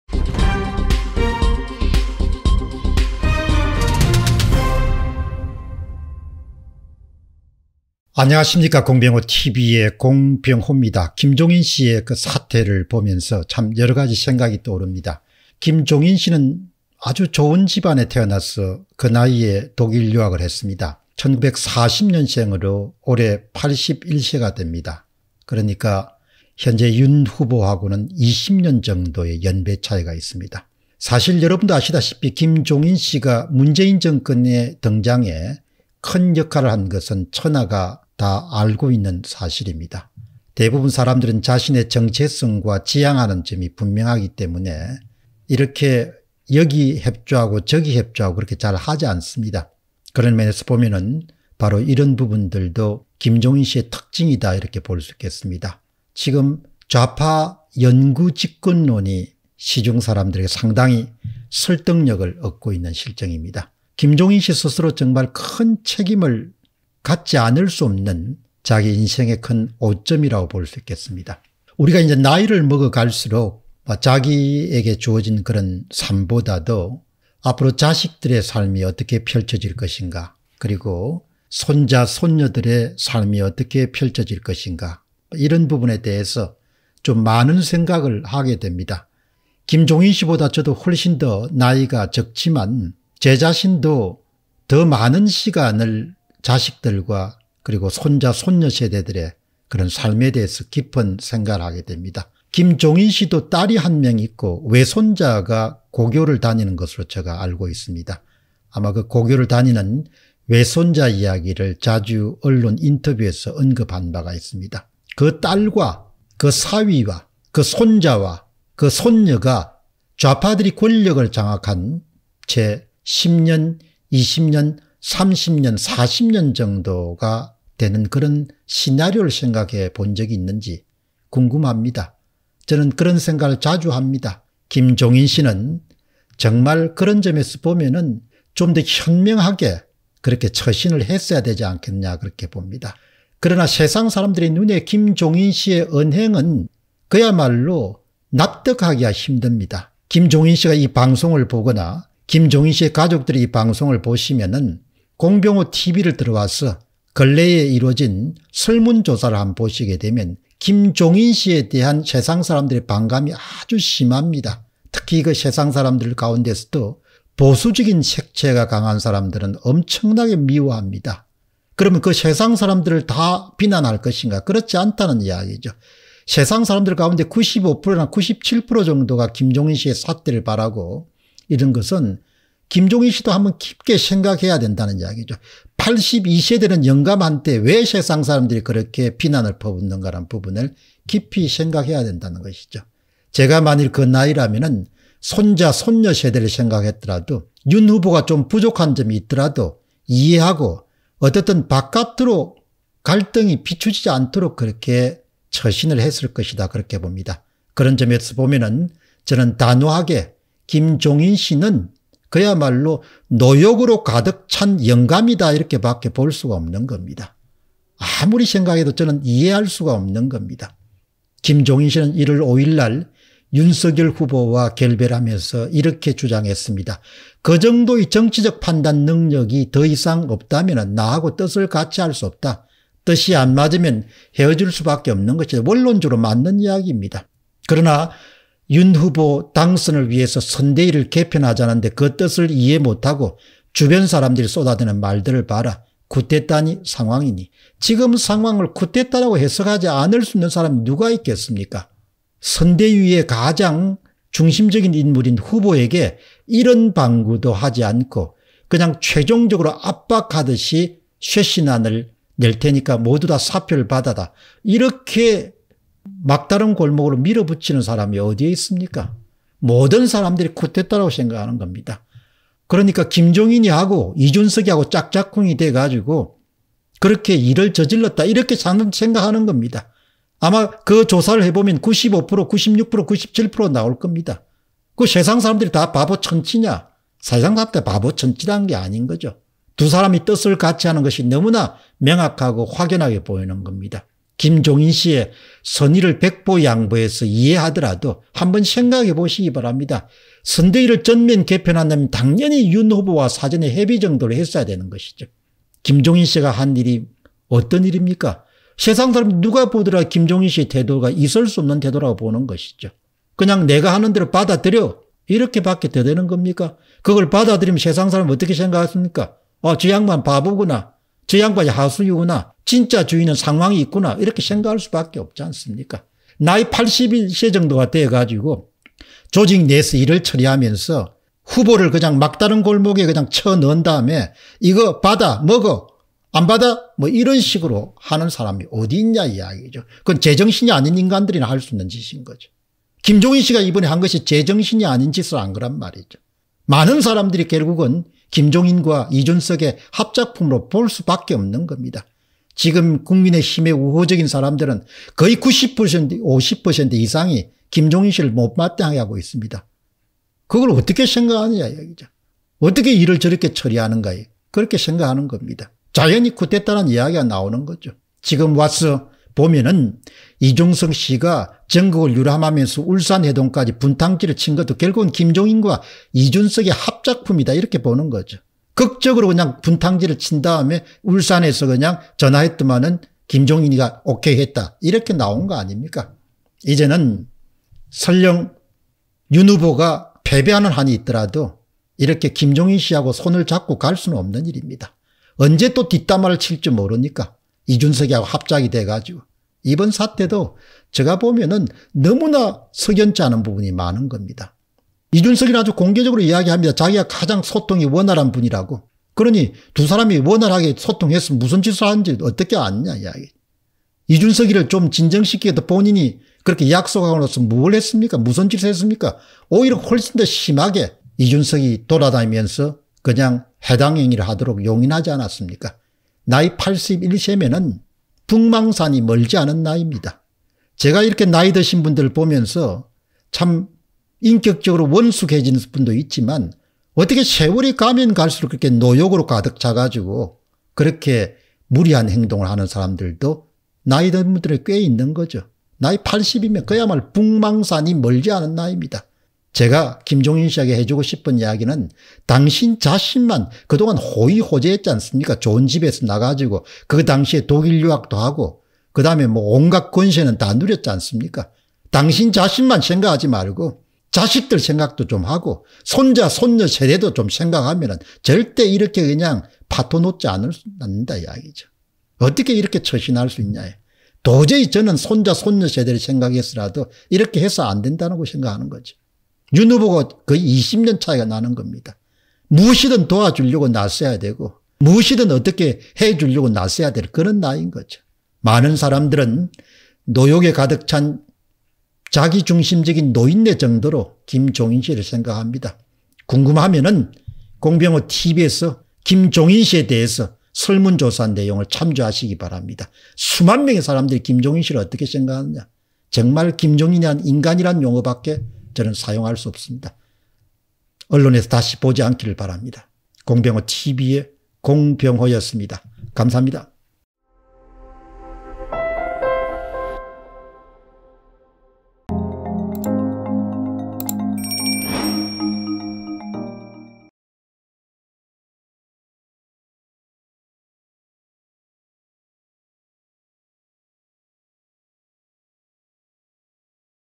당신은 자식도 없고, 손자 손녀도 없는가? / 김종인이란 인간 / 어쩌면 그렇게 노욕을 가득찼는지 / 좌파영구집권에 부역한 자로 기억될 것 [공병호TV]
Bitcoin : 3MTW849AkP2ftLeSK2teZHMmzLu7Z83s9U